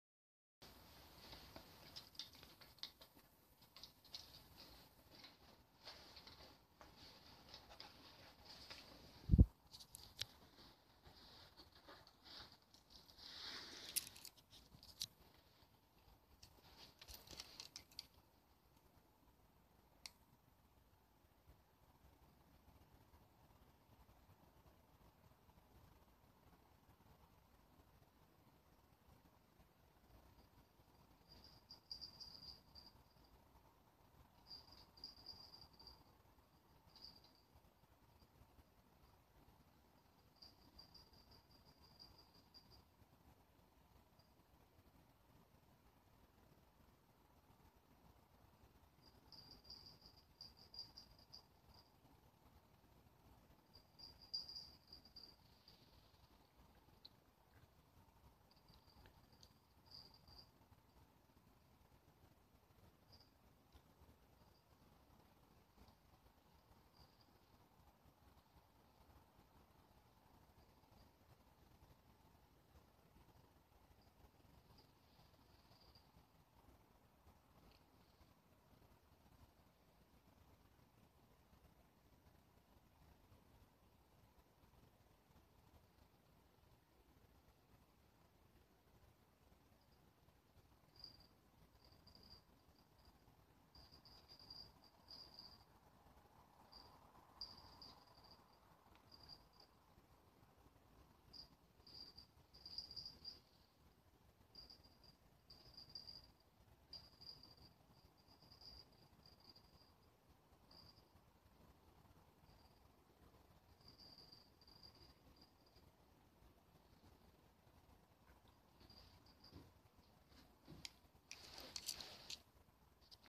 Yöleikkijä
"Hieman" huono laatuinen video.

Maikki tapaa vastustajan iltamyöhällä.